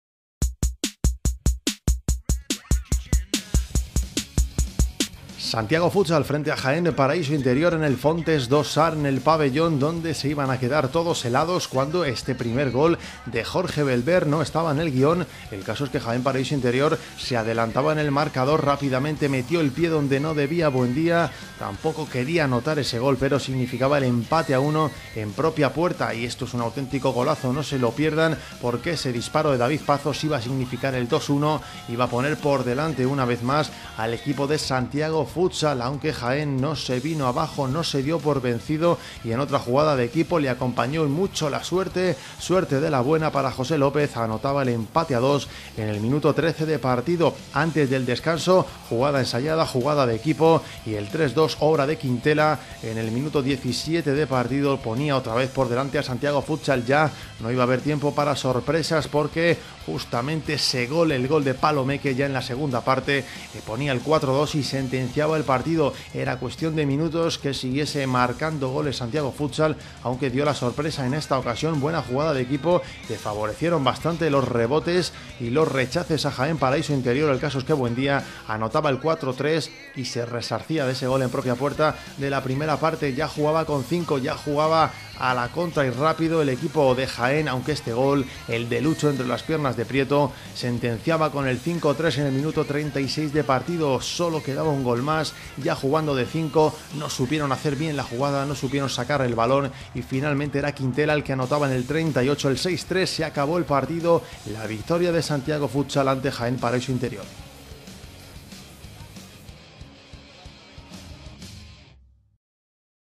Santiago Futsal 6 Vs 3 Jaen Paraiso Interior
Resumen del partido Santiago Futsal - Jaén paraíso Interior, correspondiente a la 9ª jornada de la LNFS 13/14